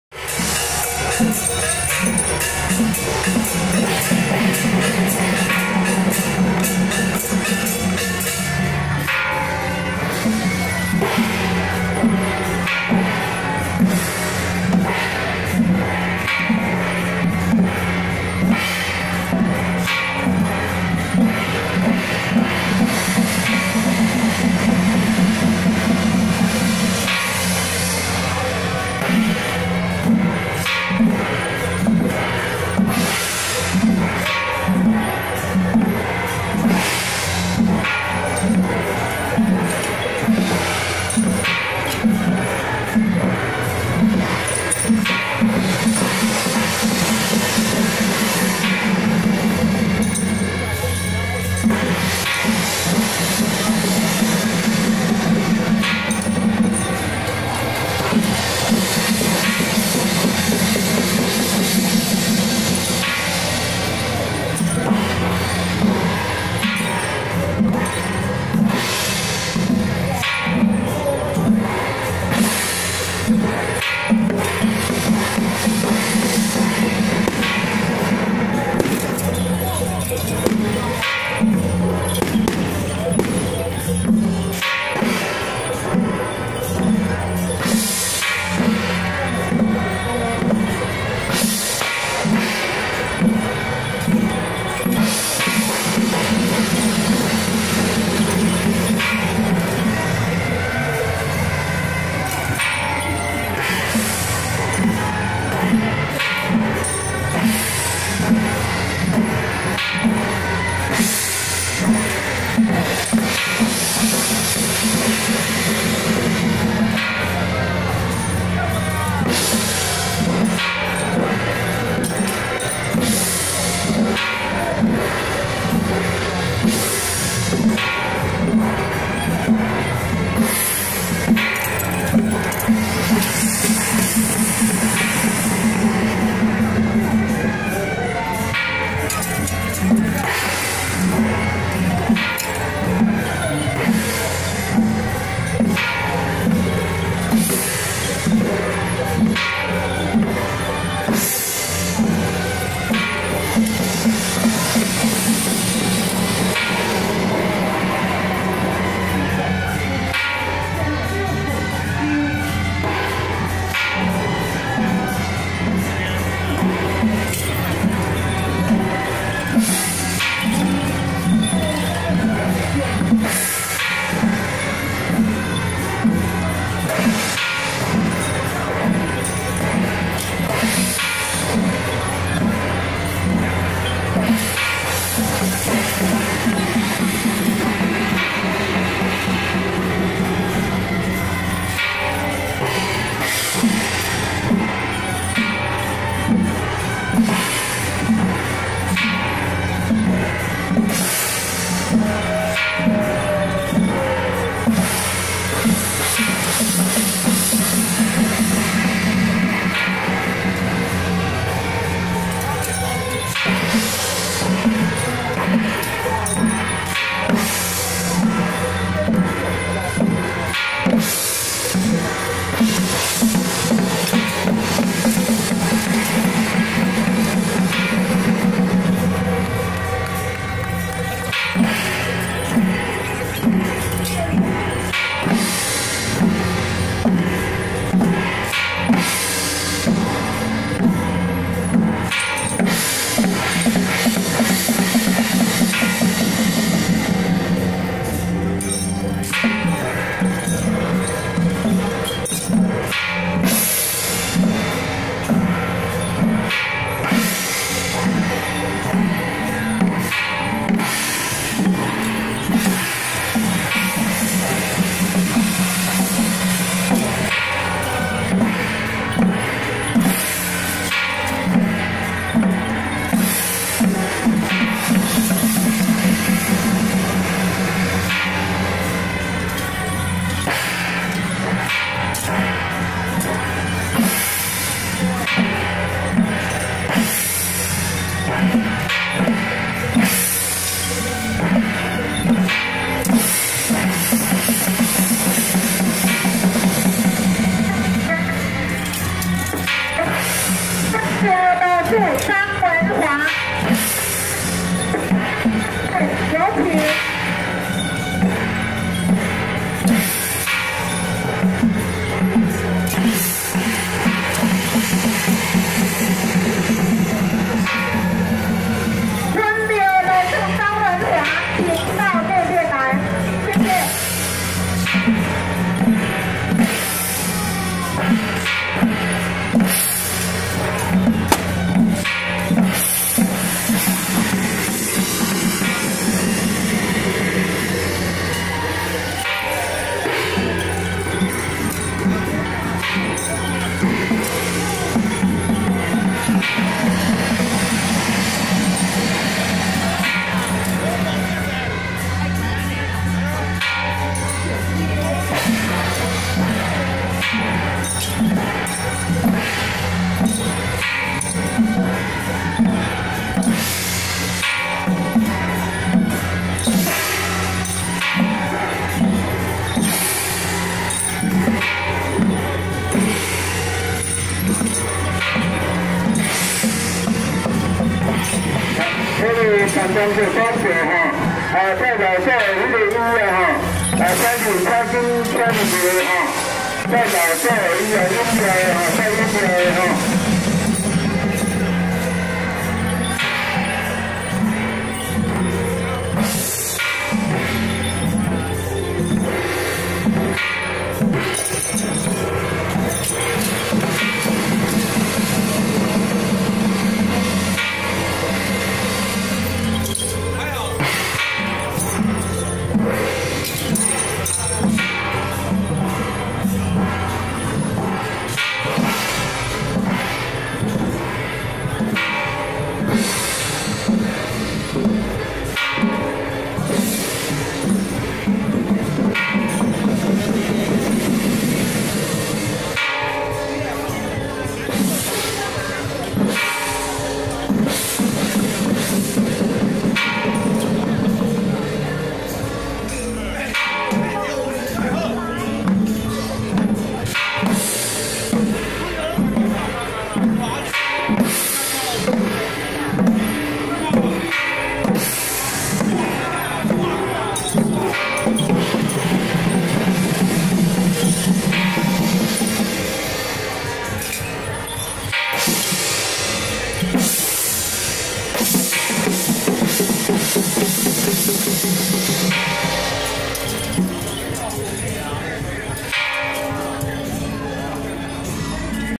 108淡水義山集應廟九年一次輪值迎老祖慶典遶境【淡水清保義-官將首】
淡水清保義-官將首